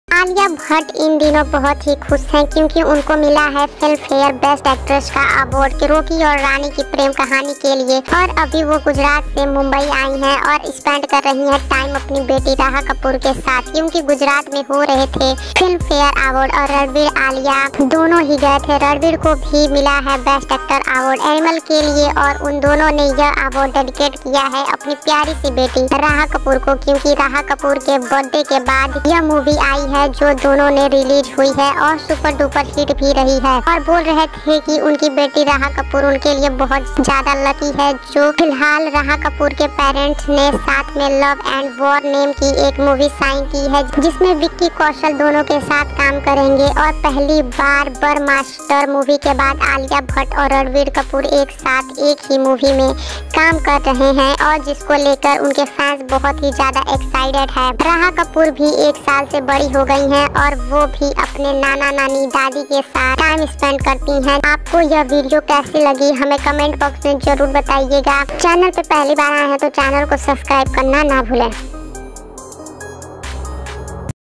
Alia bhatt with daughter Raha Kapoor spotted || enjoying private time with raha Filmfare awards 2024
Alia bhatt Ranbir kapoor with daughter Raha Kapoor going Gujarat to attend 69th Filmfare awards!